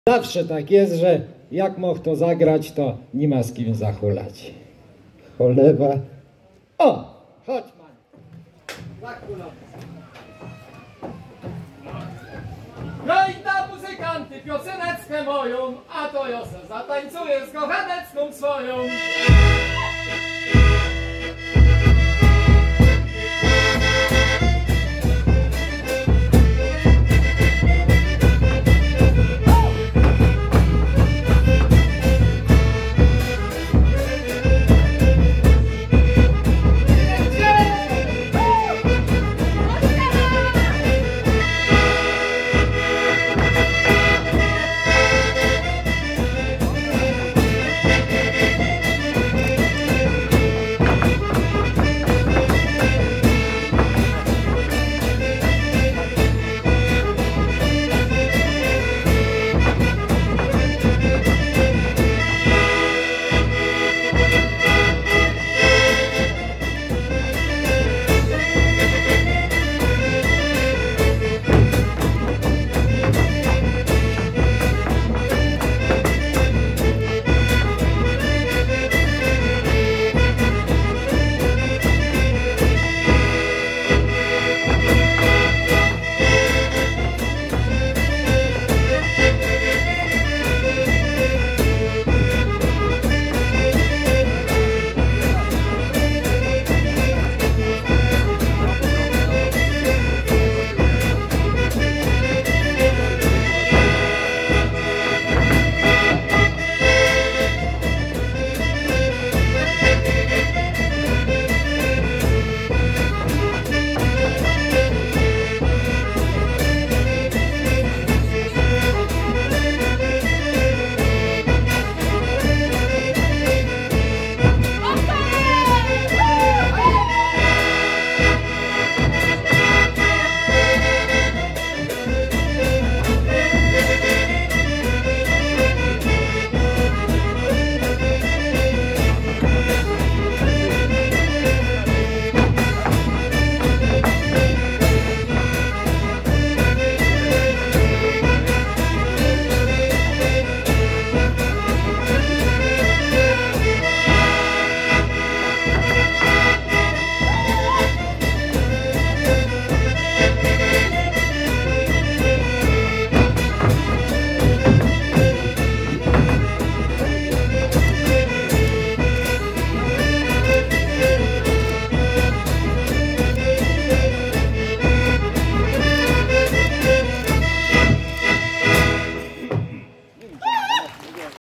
2020 09 20 Sieradz2 4
Tańczą Ewa Ziętek z Przytyka i Zbigniew Ciechowicz z Rdzowa a przygrywa kapela Józefa Kowalczyka z Sadkowic.